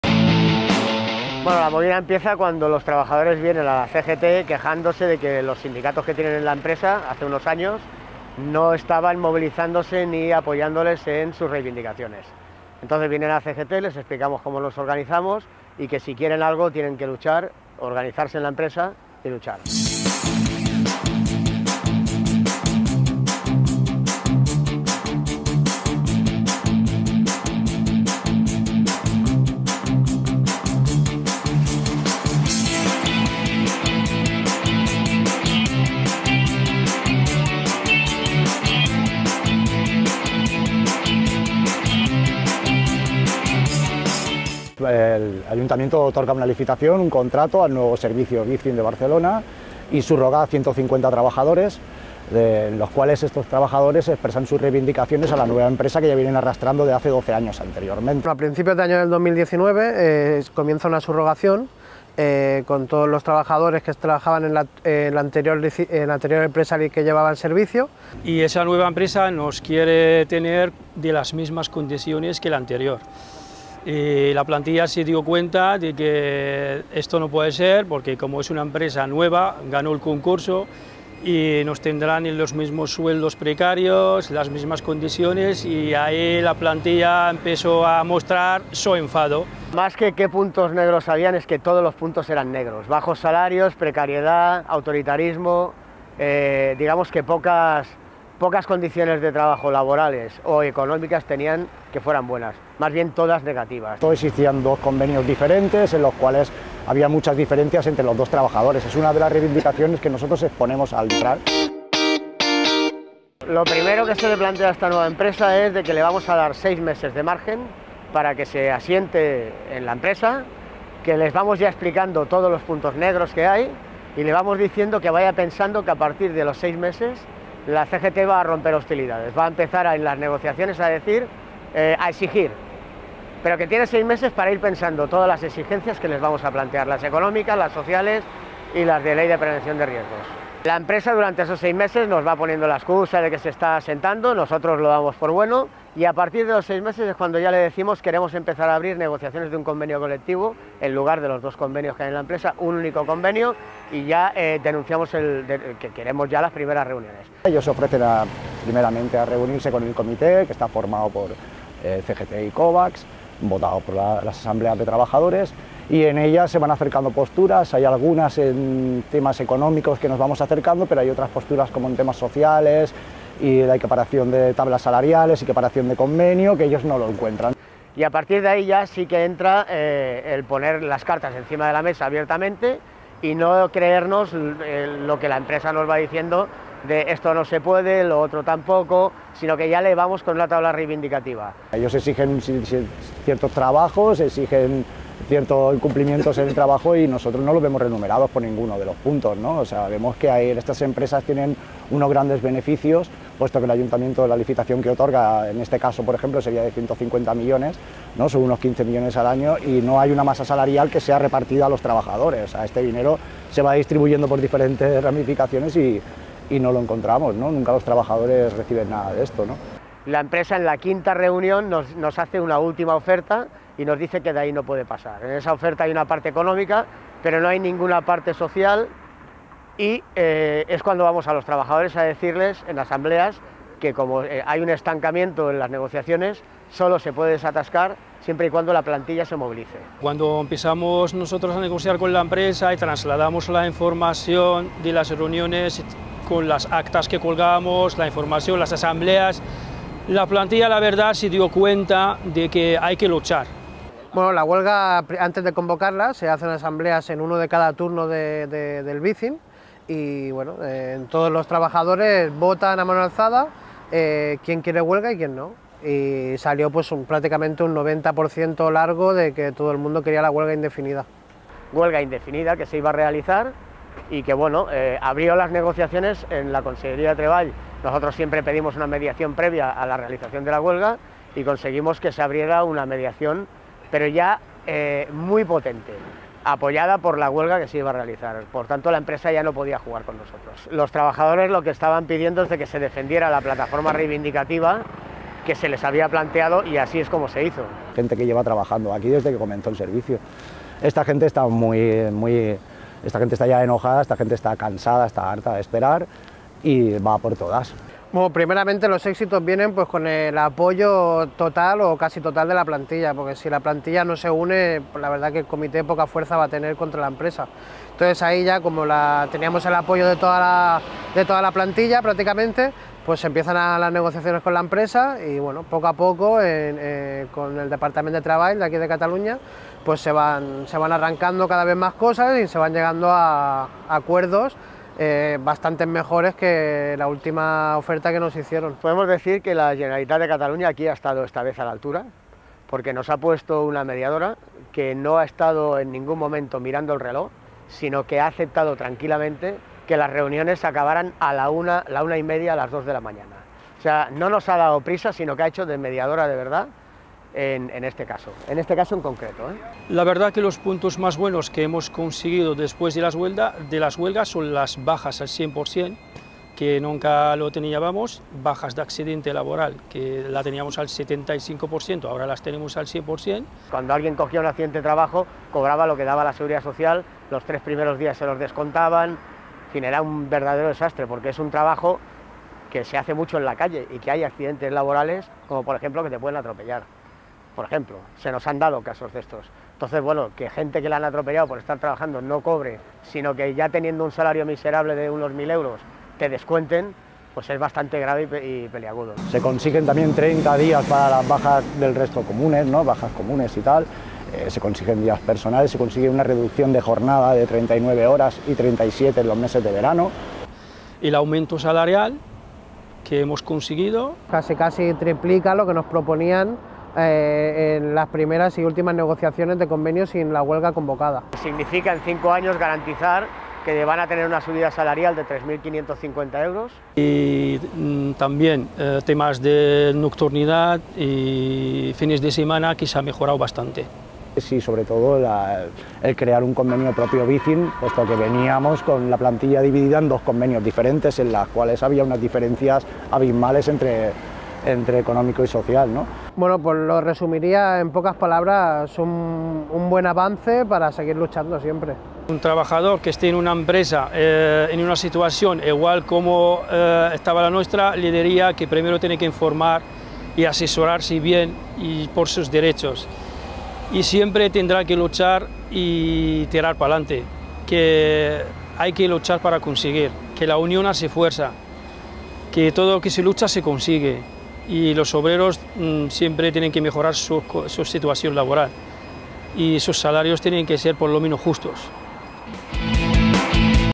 RNtvLucha Bicing Barcelona
Bicing Barcelona: La unidad, la determinación y la lucha de los trabajadores de bicing ha conseguido entre algunas otras mejoras:
    - Único Convenio (coexistían dos en el servicio)
    - Aumento salarial de 3.550 euros (a desarrollar en 5 años)
    - Se abonará el 100% de las bajas médicas
    - Se cubrirán puestos de trabajo estructurales con personal indefinido. Prohibición de contratar mediante ETT
    -Eliminación de la jornada partida y garantía de 2 días de descanso consecutivos
    - Creación de pluses de fin de semana y festivos
    - Aumento lineal del plus nocturnidad
    - Ultraactividad del convenio